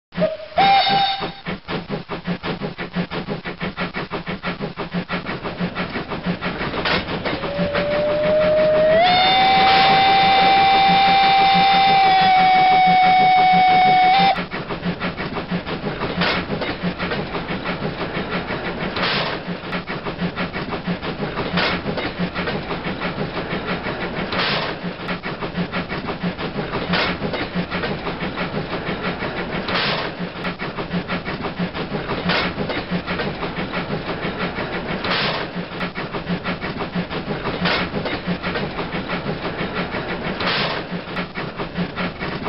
LGB Ballenberg Railway on Ceiling
This is a movie about my toy train mounted just below the ceiling.